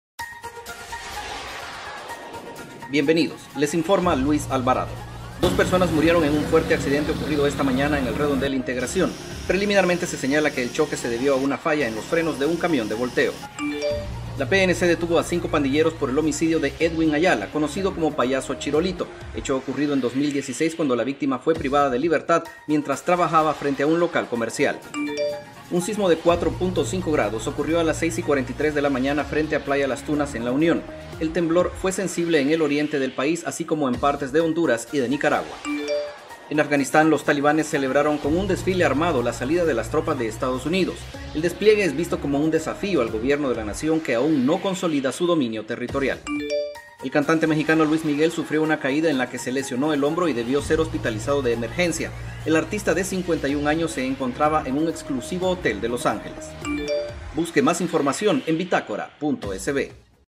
Noticias Bitácora
Dos personas mueren en fuerte accidente de tránsito.

El resumen de las noticias más importantes, desde la Sala de Redacción de Bitacora.sv